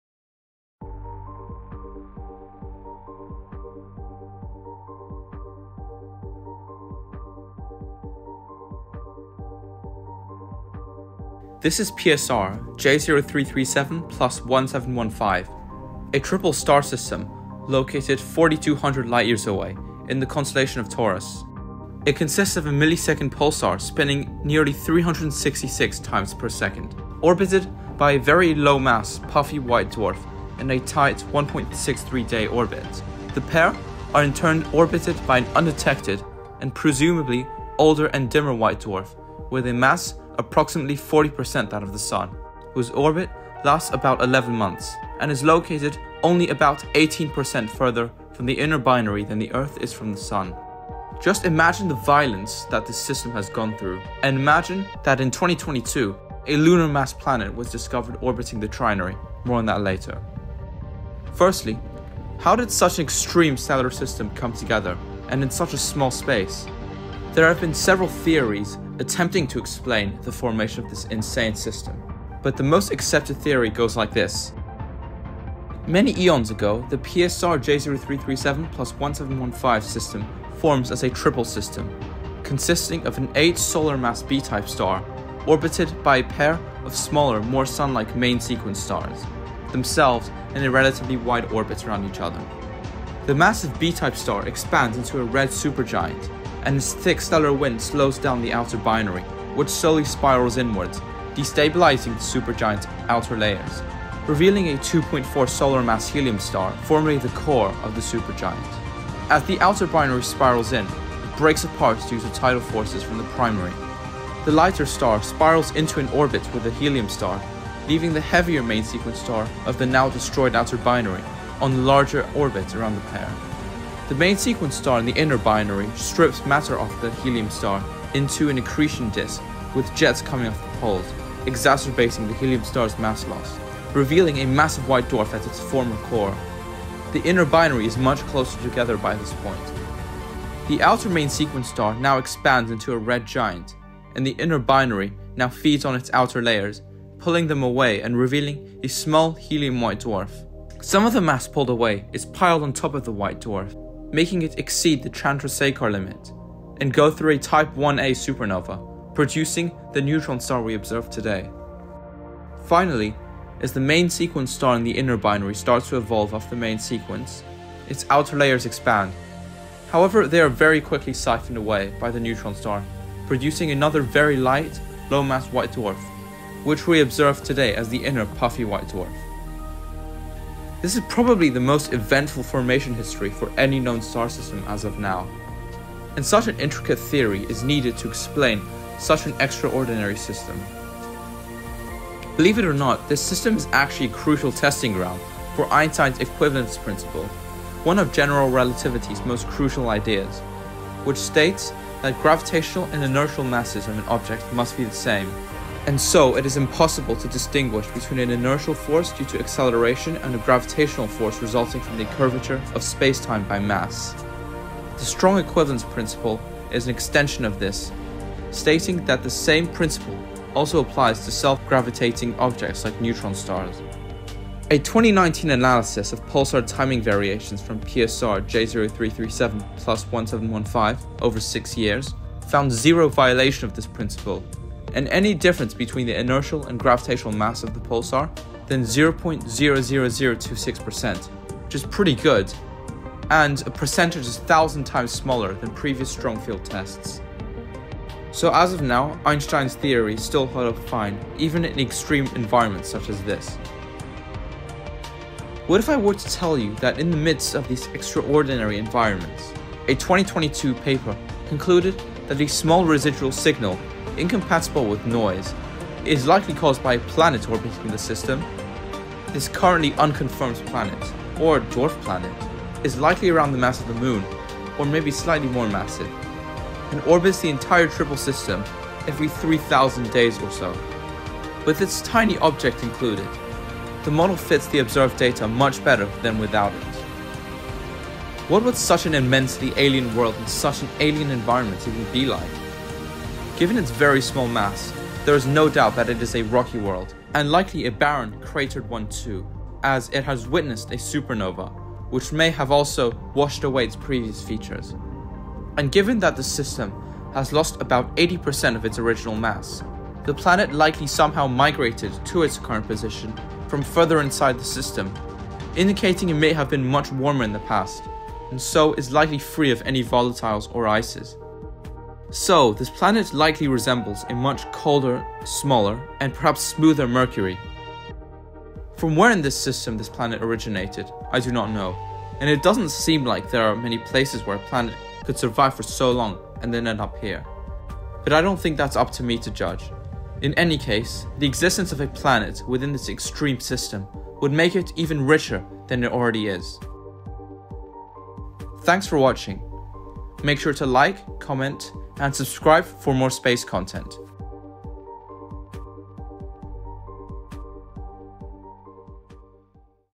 A Planet In the Strangest Star System Known?
This video is about the incredibly exotic triple star system PSR J0337+1715, located about 4200 light years away in the constellation of Taurus. It is composed of a neutron star orbited by a low-mass white dwarf, and the pair is then orbited by another white dwarf. Unsurprisingly, a very intricate formation history of the system is needed to explain its existence. The entire system is also an important testing ground for Einstein’s Equivalence Principle, a crucial idea in the theory of General Relativity.

In 2022, a small residual signal which could not be explained as noise was found to possibly be the result of a small Lunar-mass planet orbiting the triple star system every 3000 days or so, making it one of the first known circumtrinary planets, if it exists that is. This likely rocky exoplanet, or exo dwarf planet is located in one of the most alien environments we have found as of yet.

Sources:

- formation method of the system
- testing the strong equivalence principle (please read this if you are interested)

Footage from:
SpaceEngine
Celestia
various institutions such as NASA, ESA, and others which I have credited in the video

0:00 Introduction
1:15 System History
3:11 General Relativity
4:15 The Exoplanet